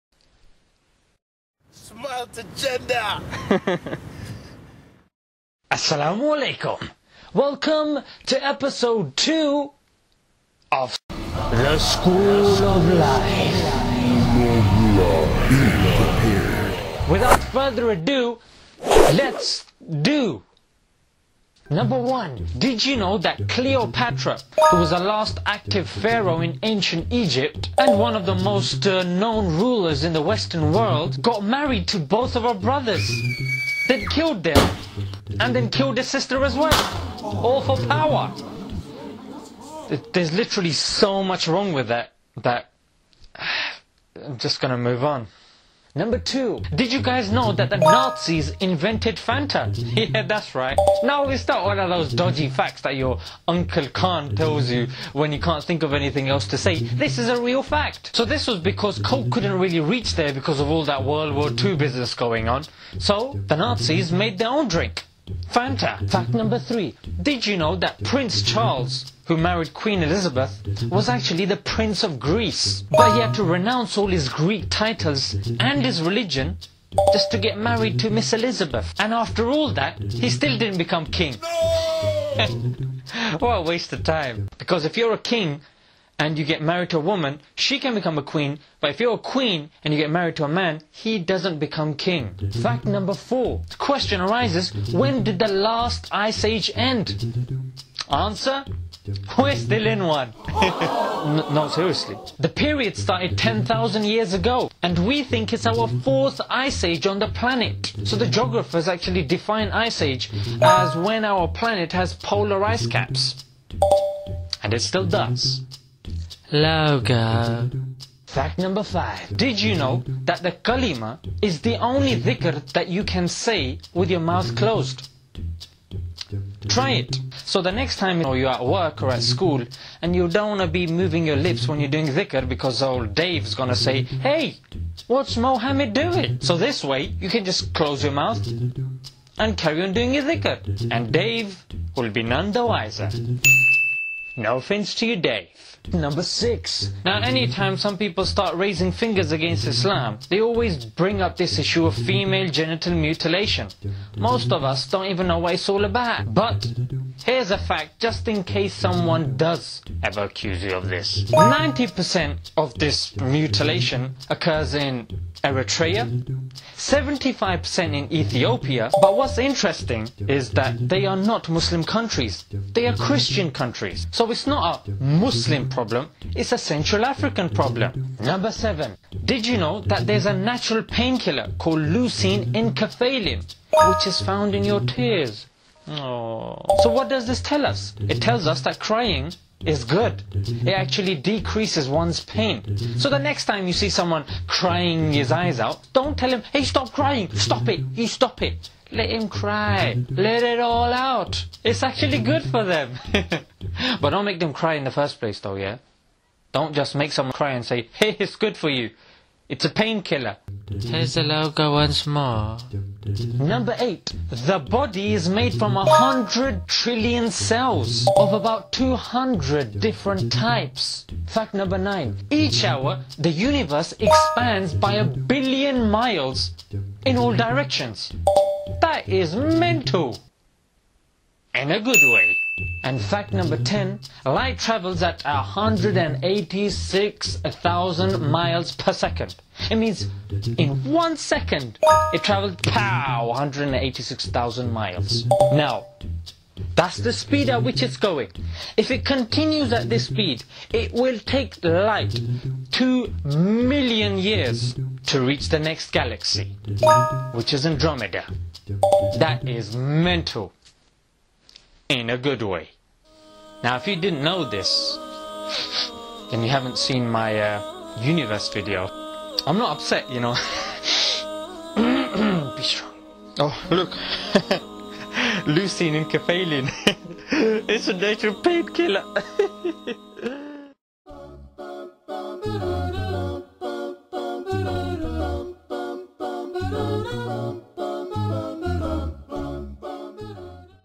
SOL Ep #2: CRYING IS GOOD FOR YOU
*mistake in fact #3. It's supposed to be Prince Philip NOT Prince Charles

School of life: 10 facts you didn't know
EPISODE 2
(featuring Cleopatra, Nazis, and the speed of light)